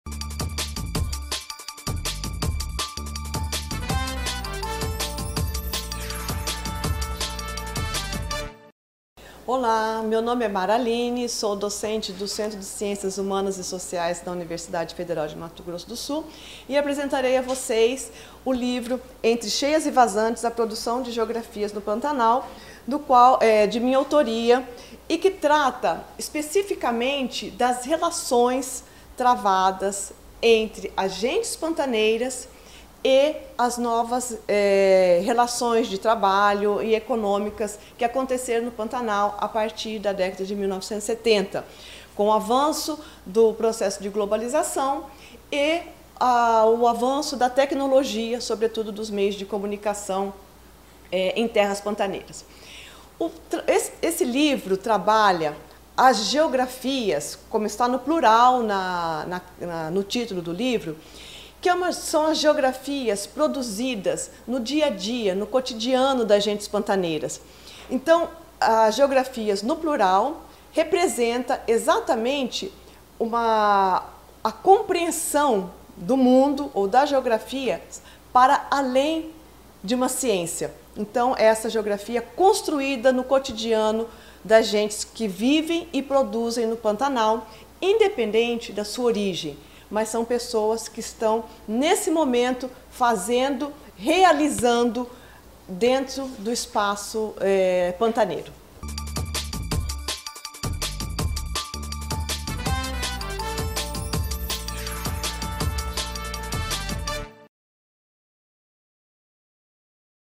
Dica de Livro - Entre cheias e vazantes
A professora Mara Aline apresenta sua obra "Entre cheias e vazantes: a produção de geografias no Pantanal". A obra foi lançada pela editora da UFMS. Acompanhe e boa leitura!